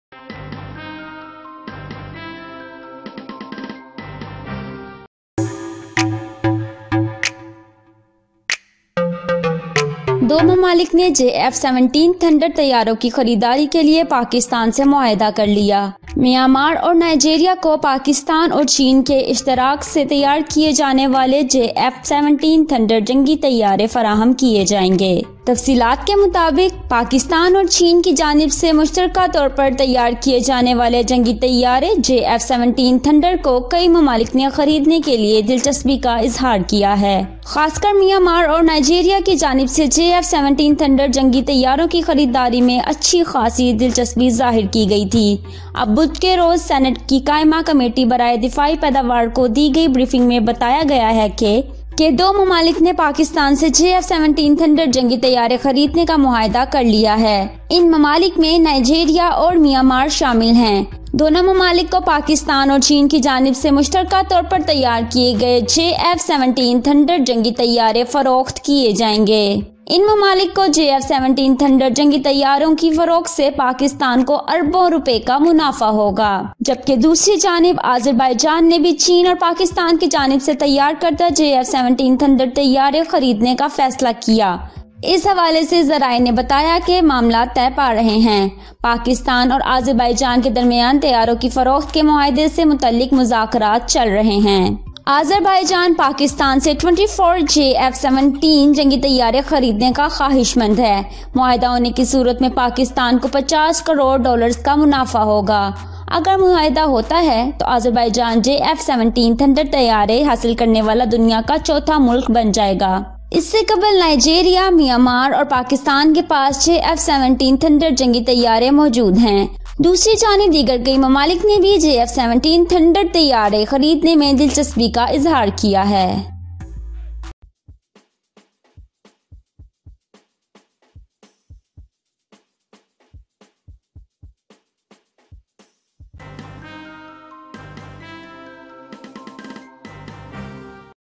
جس کا سب کو تھا انتظار وہ وقت آ ہی گیا || دو ممالک نے JF 17 کا معاہدہ پکا کر لیا
Myanmar And Nigeria JF 17 From Pakistan